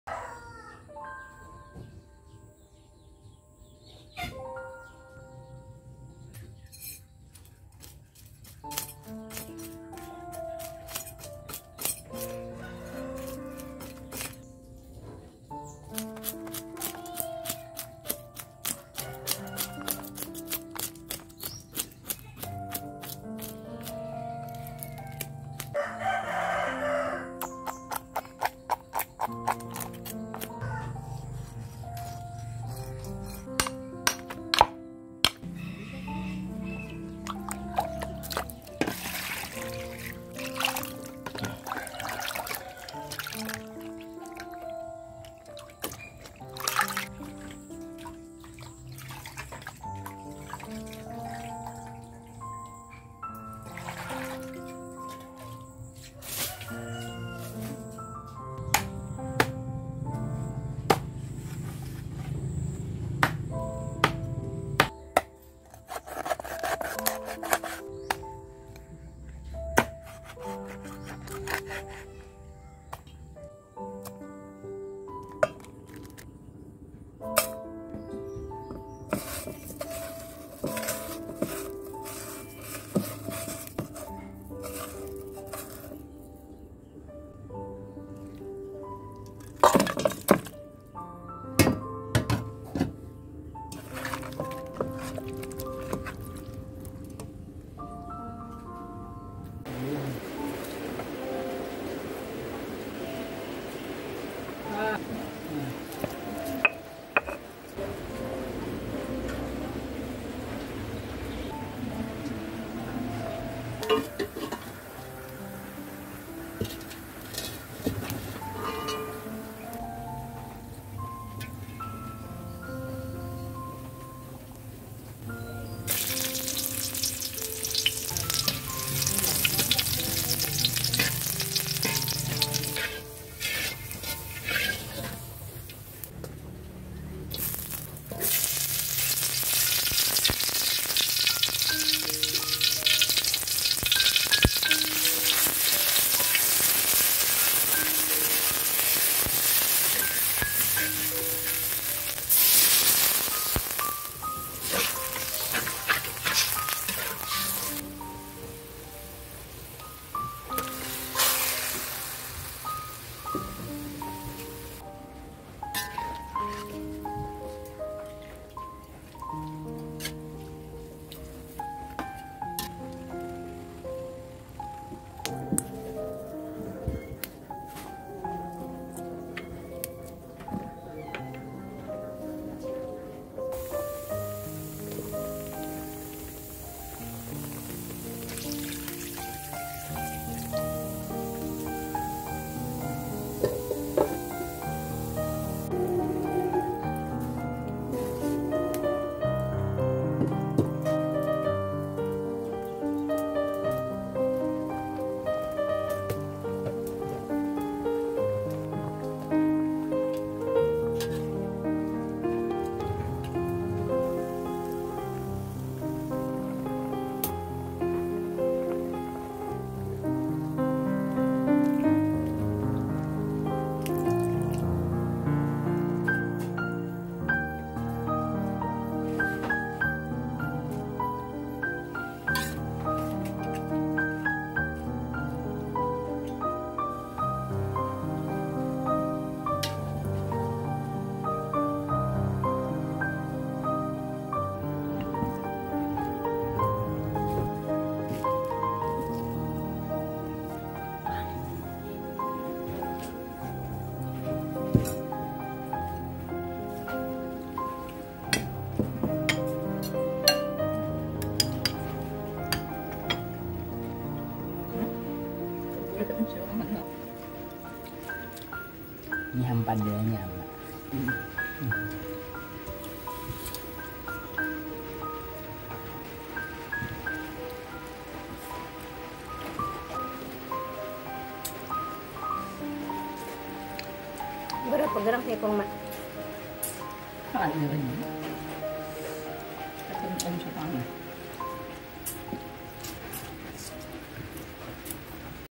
Resep Sederhana Gangan Asam Haruan Khas Banjar
Masak Gangan Asam Haruan Resep Masak Ikan Gabus Khas Banjar

Masak Sayur Asem Gabus Gangan Asam Haruan  Khas Banjar

Resep gangan asam ikan gabus ini ku dapat dari ibuku, masakan beliau cukup   sederhana dan mudah di ikuti

 bahan bumbunya

bawang merah
bawang putih
kemiri
kunyit
kencur
jahe
lengkuas
serai
terasi

Saat direbus saya menambahkan air asam jawa karena belimbing wuluh kami nggak berbuah jadi pakai air asam saja.